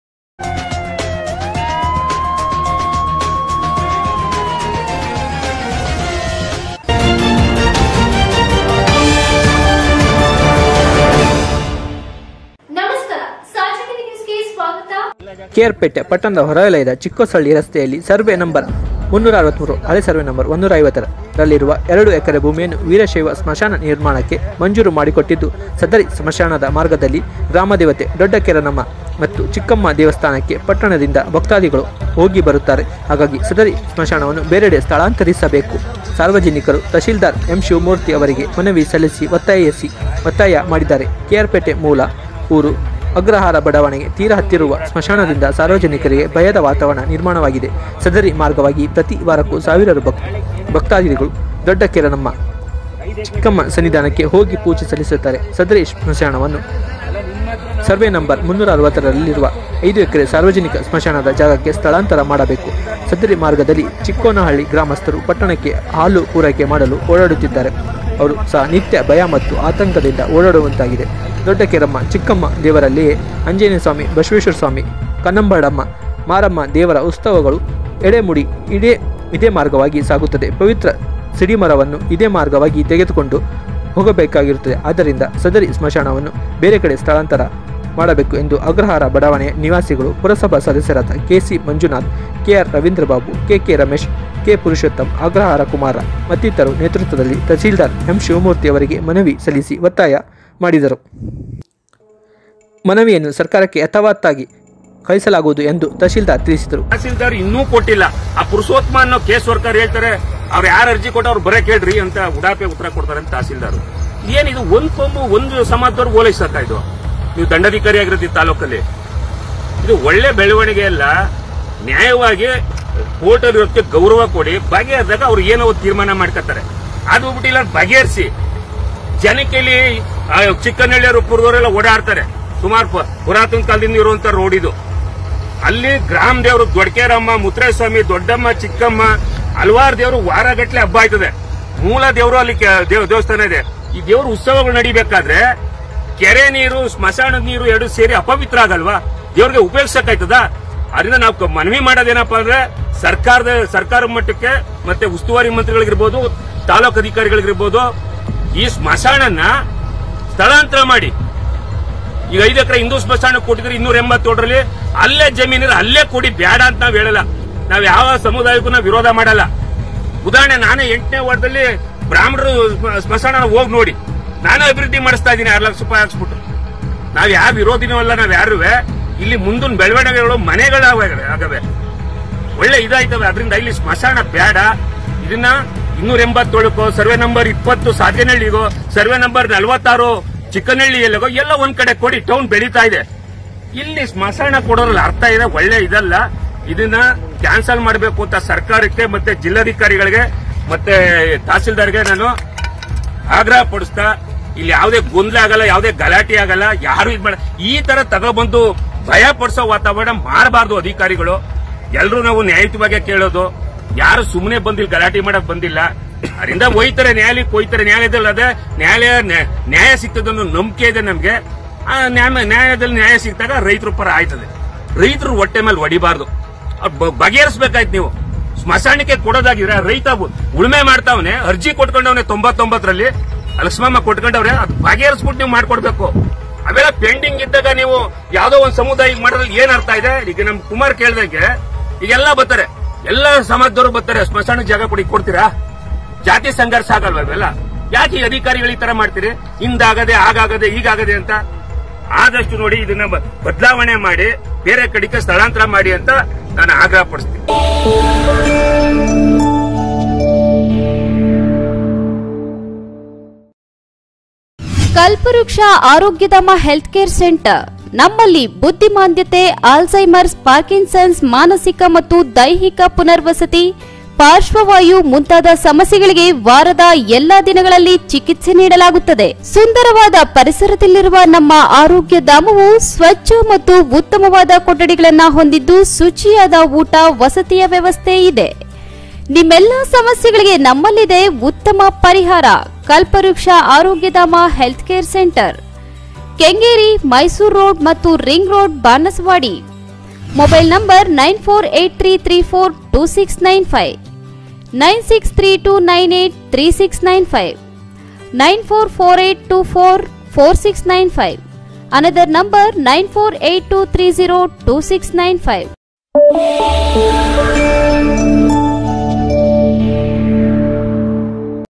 ಕೆ.ಆರ್.ಪೇಟೆ: ಪಟ್ಟಣದ ಹೊರವಲಯದ ಚಿಕ್ಕೋಸಹಳ್ಳಿ ರಸ್ತೆಯಲ್ಲಿರುವ ಸರ್ವೆ ನಂ. ೩೬೩( ಹಳೆ ಸ.ನಂ.೧೫೬)ರಲ್ಲಿರುವ
ಕೆ.ಆರ್.ಪೇಟೆ: ಪಟ್ಟಣದ ಹೊರವಲಯದ ಚಿಕ್ಕೋಸಹಳ್ಳಿ ರಸ್ತೆಯಲ್ಲಿರುವ ಸರ್ವೆ ನಂ. ೩೬೩( ಹಳೆ ಸ.ನಂ.೧೫೬)ರಲ್ಲಿರುವ ಎರಡು ಎಕರೆ ಭೂಮಿಯನ್ನು ವೀರಶೈವ ಸ್ಮಶಾನ ನಿರ್ಮಾಣಕ್ಕೆ ಮಂಜೂರು ಮಾಡಿಕೊಟ್ಟಿದ್ದು ಸದರಿ ಸ್ಮಶಾನದ ಮಾರ್ಗದಲ್ಲಿ ಗ್ರಾಮದೇವತೆ ದೊಡ್ಡಕೇರಮ್ಮ ಮತ್ತು ಚಿಕ್ಕಮ್ಮ ದೇವಸ್ಥಾನಕ್ಕೆ ಪಟ್ಟಣದಿಂದ ಭಕ್ತಾಧಿಗಳು ಹೋಗಿ ಬರುತ್ತಾರೆ ಹಾಗಾಗಿ ಸದರಿ ಸ್ಮಶಾನವನ್ನು ಬೇರೆಡೆ ಸ್ಥಳಾಂತರಿಸಬೇಕು ಸಾರ್ವಜನಿಕರು ತಹಸೀಲ್ದಾರ್ ಎಂ.ಶಿವಮೂರ್ತಿ ಅವರಿಗೆ ಮನವಿ ಸಲ್ಲಿಸಿ ಒತ್ತಾಯ ಮಾಡಿದ್ದಾರೆ. ಕೆ.ಆರ್.ಪೇಟೆ ಮೂಲ ಊರು ಅಗ್ರಹಾರ ಬಡಾವಣೆಗೆ ತೀರ ಹತ್ತಿರುವ ಸ್ಮಶಾನದಿಂದ ಸಾರ್ವಜನಿಕರಿಗೆ ಭಯದ ವಾತಾವರಣ ನಿರ್ಮಾಣವಾಗಿದೆ. ಸದರಿ ಮಾರ್ಗವಾಗಿ ಪ್ರತಿ ವಾರವೂ ಸಾವಿರಾರು ಭಕ್ತಾಧಿಗಳು ದೊಡ್ಡಕೇರಮ್ಮ-ಚಿಕ್ಕಮ್ಮ ಸನ್ನಿಧಾನಕ್ಕೆ ಹೋಗಿ ಪೂಜೆ ಸಲ್ಲಿಸುತ್ತಾರೆ. ಸದರಿ ಸ್ಮಶಾನವನ್ನು ಸ.ನಂ.೩೬೨ರಲ್ಲಿರುವ ೫ಎಕರೆ ಸಾರ್ವಜನಿಕ ಸ್ಮಶಾನದ ಜಾಗಕ್ಕೆ ಸ್ಥಳಾಂತರ ಮಾಡಬೇಕು. ಸದರಿ ಮಾರ್ಗದಲ್ಲಿ ಚಿಕ್ಕೋಸಹಳ್ಳಿ ಹಾಗೂ ಚಿಕ್ಕೋನಹಳ್ಳಿ ಗ್ರಾಮಸ್ಥರು ಪಟ್ಟಣಕ್ಕೆ ಹಾಲು ಪೂರೈಕೆ ಮಾಡಲು ಓಡಾಡುತ್ತಿದ್ದಾರೆ. ಅವರು ಸಹ ನಿತ್ಯ ಭಯ ಮತ್ತು ಆತಂಕದಿAದ ಓಡಾಡುವಂತಾಗಿದೆ. ದೊಡ್ಡಕೇರಮ್ಮ-ಚಿಕ್ಕಮ್ಮ ದೇವರಲ್ಲದೇ ಆಂಜನೇಯಸ್ವಾಮಿ, ಬಸವೇಶ್ವರ ಸ್ವಾಮಿ, ಕನ್ನಂಬಾಡಮ್ಮ, ಮಾರಮ್ಮ ದೇವರ ಉತ್ಸವಗಳು, ಯಡೆ-ಮುಡಿ ಇದೇ ಮಾರ್ಗವಾಗಿ  ಸಾಗುತ್ತದೆ. ಪವಿತ್ರ ಸಿಡಿಮರವನ್ನು ಇದೇ ಮಾರ್ಗವಾಗಿ ತೆಗೆದುಕೊಂಡು ಹೋಗಬೇಕಾಗಿರುತ್ತದೆ ಆದ್ದರಿಂದ ಸದರಿ ಸ್ಮಶಾನವನ್ನು ಬೇರೆಡೆ ಸ್ಥಳಾಂತರ ಮಾಡಬೇಕು ಎಂದು ಅಗ್ರಹಾರ ಬಡಾವಣೆಯ ನಿವಾಸಿಗಳು ಪುರಸಭಾ ಸದಸ್ಯರಾದ ಕೆ.ಸಿ.ಮಂಜುನಾಥ್, ಕೆ.ಆರ್.ರವೀಂದ್ರಬಾಬು, ಕೆ.ಕೆ.ರಮೇಶ್, ಕೆ.ಪುರುಷೋತ್ತಮ್, ಅಗ್ರಹಾರ ಕುಮಾರ್ ಮತ್ತಿತರರ ನೇತೃತ್ವದಲ್ಲಿ ತಹಸೀಲ್ದಾರ್ ಎಂ.ಶಿವಮೂರ್ತಿ ಅವರಿಗೆ ಮನವಿ ಸಲ್ಲಿಸಿ ಒತ್ತಾಯ ಮಾಡಿದರು. ಮನವಿ ಸ್ವೀಕರಿಸಿದ ತಹಸೀಲ್ದಾರ್ ಎಂ.ಶಿವಮೂರ್ತಿ ಅವರು ಮಾತನಾಡಿ ನಿಮ್ಮ ಮನವಿಯನ್ನು ಸರ್ಕಾರಕ್ಕೆ ಯಥಾವತ್ ಕಳುಹಿಸಿಕೊಡಲಾಗುವುದು ಎಂದು ತಹಸೀಲ್ದಾರ್ ತಿಳಿಸಿದರು.
ವರದಿ. ಪ್ರತಾಪ್. ಎ.ಬಿ
*ಮಳವಳ್ಳಿ